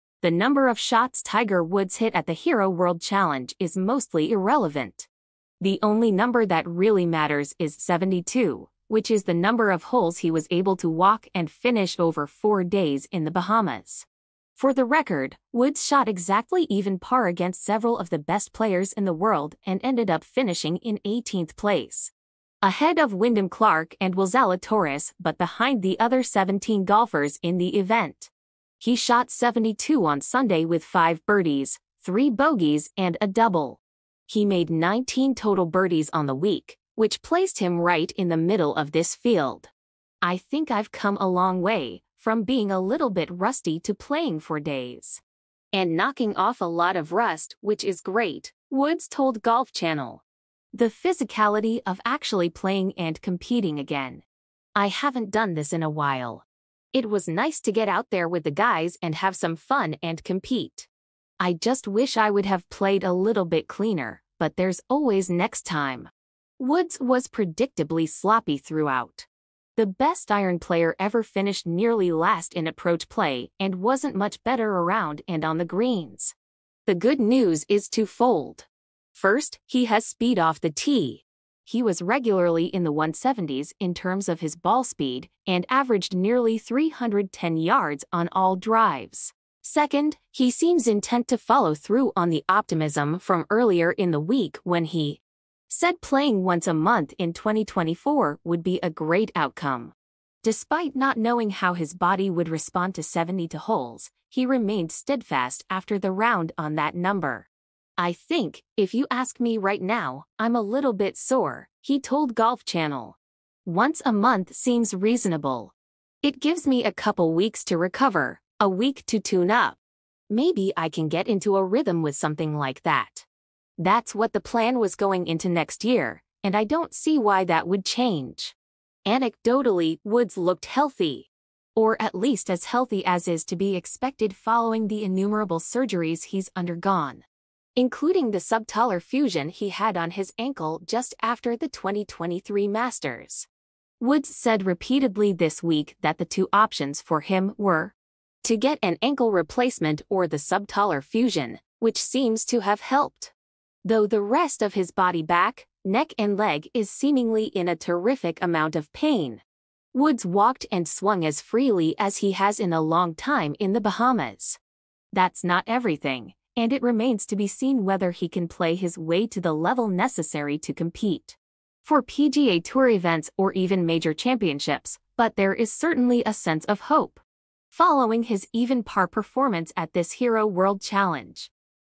Tiger Woods finishes even par at 2023 Hero World Challenge to cap optimistic performance in return
The number of shots Tiger Woods hit at the Hero World Challenge is mostly irrelevant. The only number that really matters is 72, which is the number of holes he was able to walk and finish over four days in the Bahamas.

For the record, Woods shot exactly even par against several of the best players in the world and ended up finishing in 18th place, ahead of Wyndham Clark and Will Zalatoris but behind the other 17 golfers in the event. He shot 72 on Sunday with five birdies, three bogeys and a double. He made 19 total birdies on the week, which placed him right in the middle of this field.